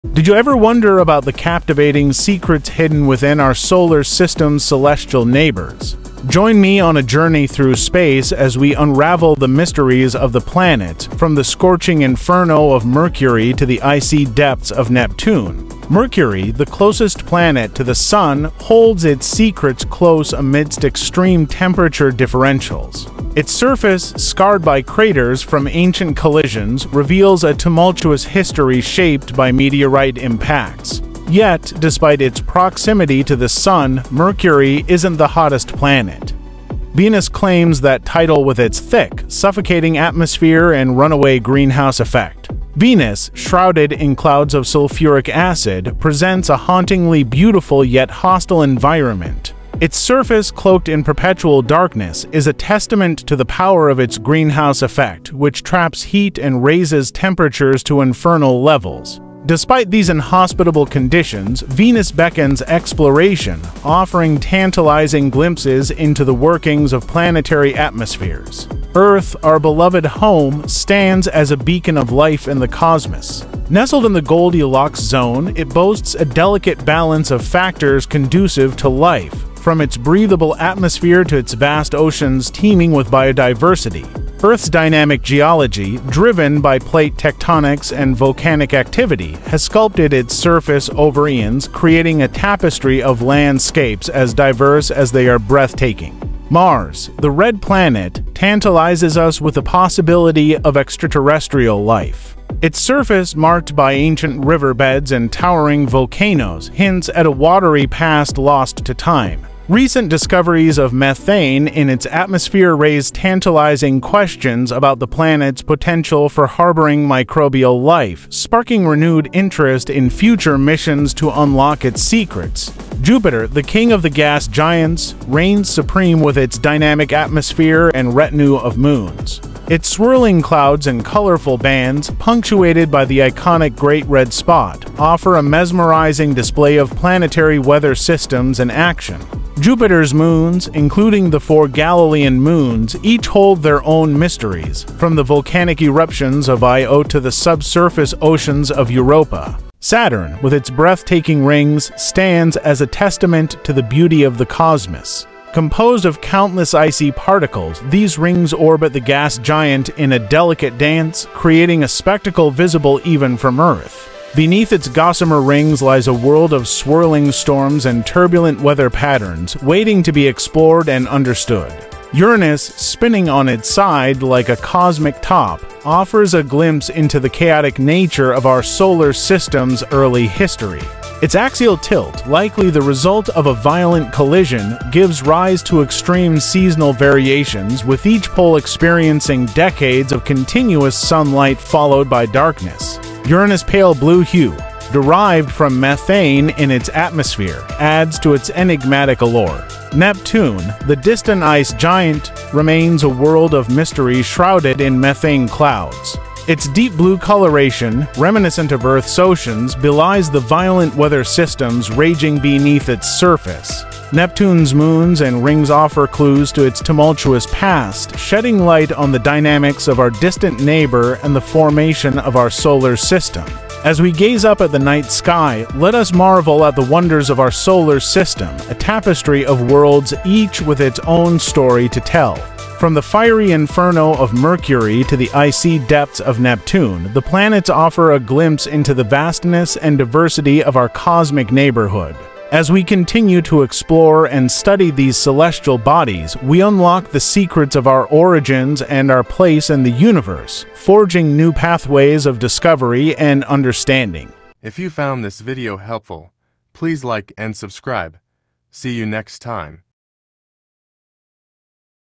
What SECRETs Lie Within Our SOLAR SYSTEM?
Embark on a cosmic journey to uncover the fascinating secrets of our solar system’s planets. From Mercury’s extreme temperatures to Neptune’s violent winds, each planet offers unique insights into the wonders of the universe. Discover the mysteries that lie within our celestial neighbors and gain a deeper understanding of our place in the cosmos.